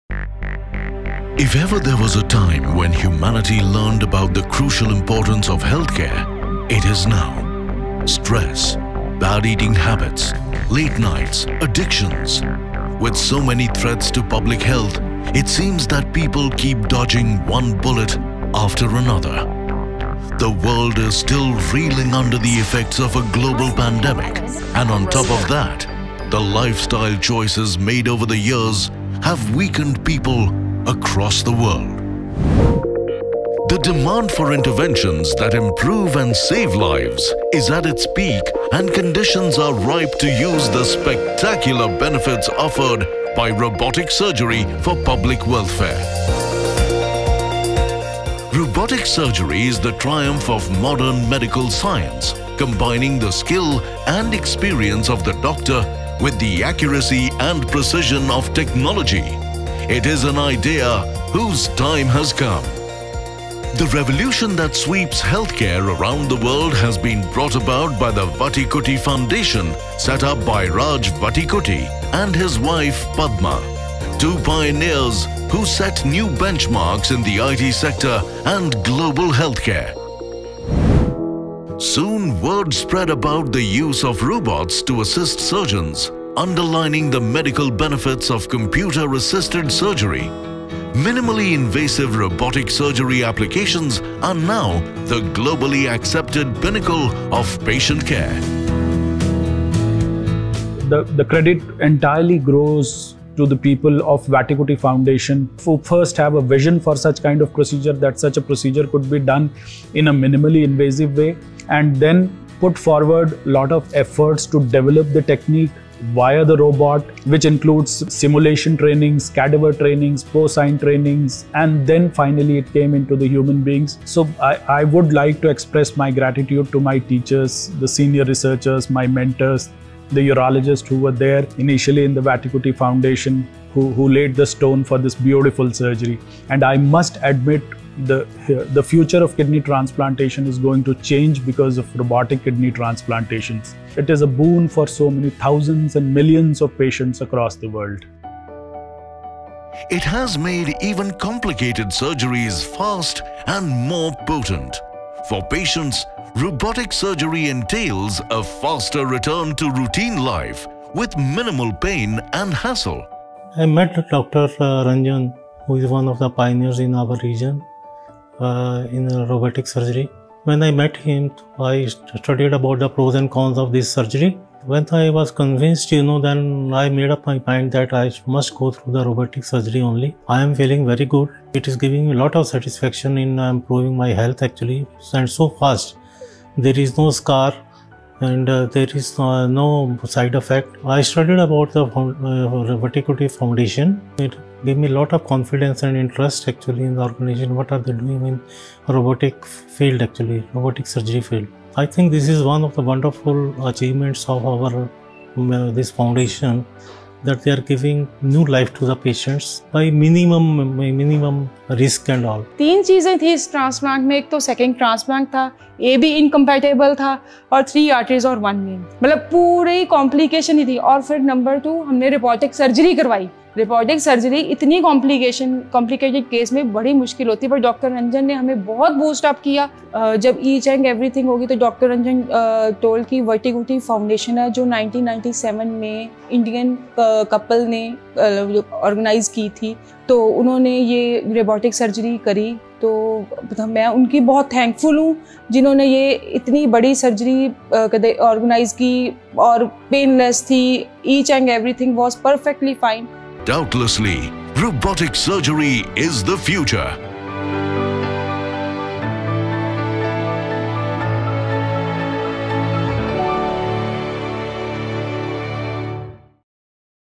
Dr. Priyadarshi Ranjan on the Vattikuti Foundation
Fortis Hospital Mohali, India Robotic Transplant Surgeon Dr. Priyadarshi Ranjan and two of his robotic kidney transplant patients talk about what the Vattikuti Foundation's efforts to bring robotic surgery to the world means to them.  4:28